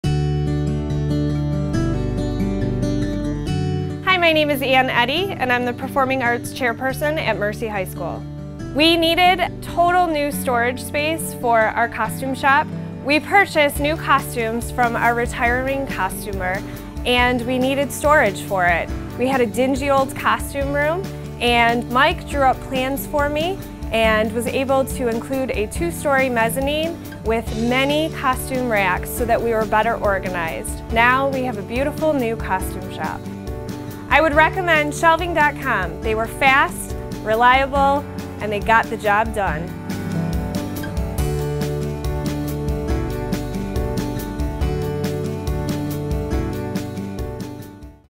Mercy High School Testimonial | Project Spotlight
Shelving.com worked with Mercy High School's drama department to double their storage space with a mezzanine, rivet shelving, and wire shelving. The result is an organized and efficient space with tons of room for costumes and props.

See what we can do for your space - request a quote on our website today!